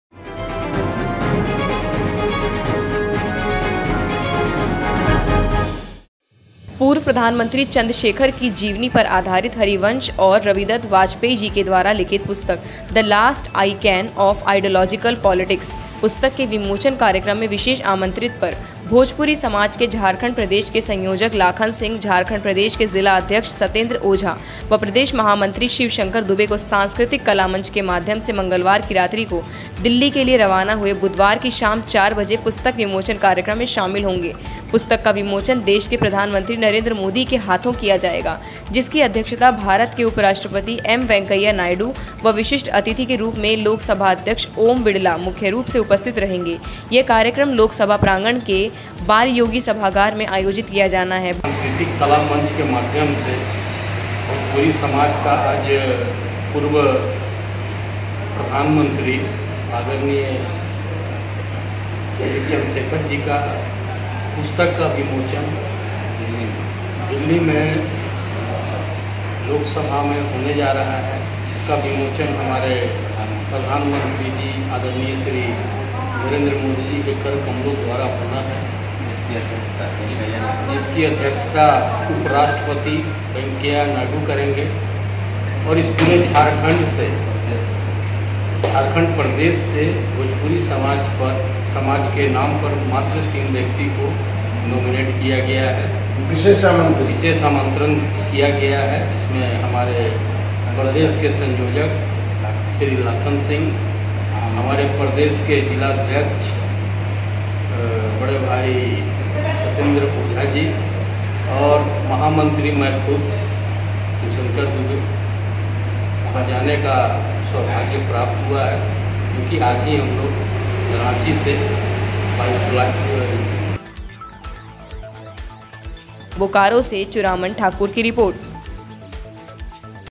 KHABAR 24 NEWS/बोकारो-पूर्व प्रधानमंत्री चंद्रशेखर के जीवनी पर आधारित पुस्तक The last I can of IDEIO
पूर्व प्रधानमंत्री चंद्रशेखर के जीवनी पर आधारित पुस्तक The last I can of IDEIOLOGICAL  politics  पुस्तक के विमोचन में  विशेष आमंत्रण पर हुए दिल्ली रवाना।

पूर्व प्रधानमंत्री चंद्रशेखर की जीवनी पर आधारित हरिवंश और रवि दत वाजपेई जी के द्वारा लिखित पुस्तक The last I can of IDElOLOGICAL politics पुस्तक के विमोचन कार्यक्रम में  विशेष आमंत्रित पर भोजपुरी समाज के झारखंड प्रदेश के संयोजक लाखन सिंह, झारखंड प्रदेश के जिला अध्यक्ष सतेन्द्र ओझा व प्रदेश महामंत्री शिव शंकर दुबे को सांस्कृतिक कला मंच के माध्यम से मंगलवार की रात्रि को दिल्ली के लिए रवाना हुए जो बुधवार की शाम 4बजे पुस्तक विमोचन कार्यक्रम में शामिल होंगे। पुस्तक का विमोचन देश के प्रधानमंत्री नरेन्द्र मोदी के हाथों किया जायेगा जिसकी अध्यक्षता भारत के उपराष्ट्रपति एम वैकंया नायडू  व विशिष्ट अतिथि के रूप में लोकसभाध्यक्ष ओम बिड़ला, मुख्य रूप से उपस्थित रहेंगे यह कार्यक्रम लोकसभा प्रांगण के बालयोगी सभागार मे आयोजित किया जाना है।

बोकारो से चुरामन ठाकुर की रिपोर्ट